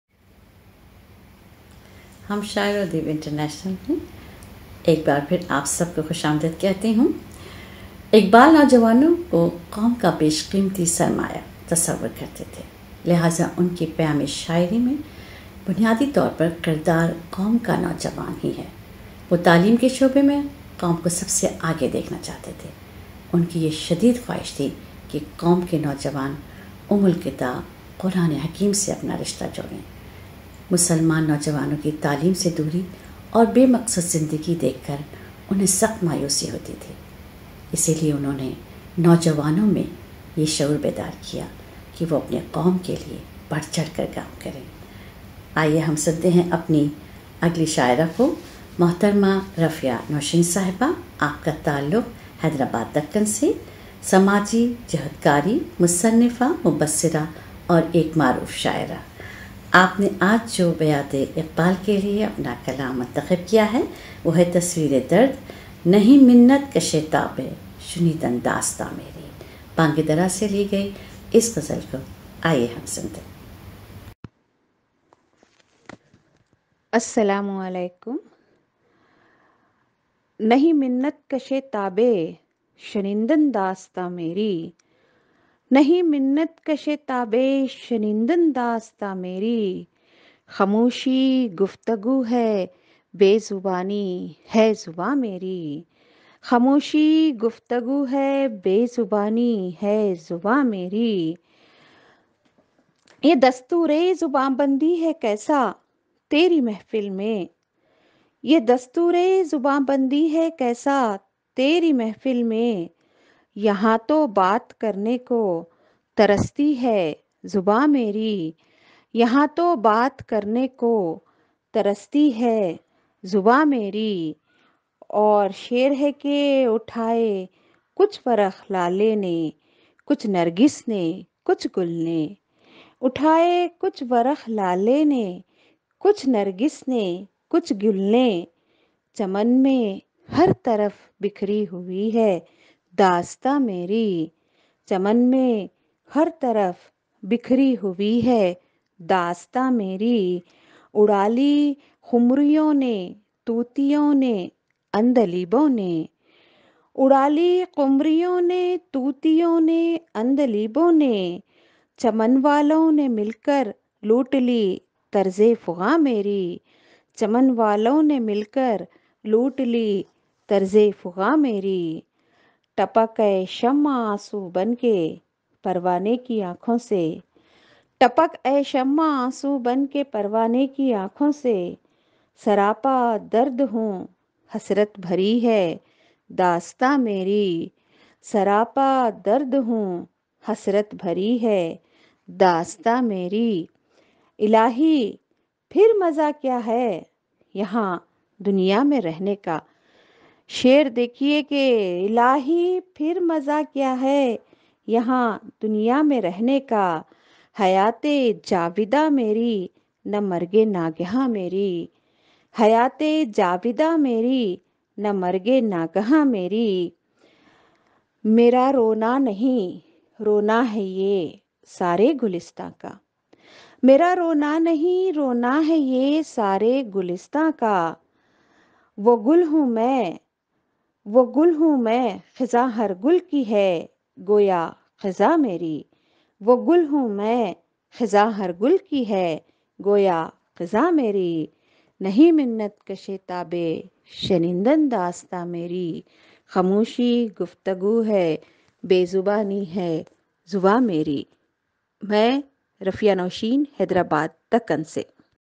Rafia Nosheen 4th Dec 2022 مشاعرہ بیاد اقبال زیر انتظام ہم شاعر و ادیب انٹرنیشنل
اآدبی تنظیم ہم شاعر و ادیب انٹرنیشنل کے زیرِ اہتمام4دسمبر 2022 بروز اتوار بیاد اقابال کے حوالے سے مشاعرے کا انعقاد کیا گیا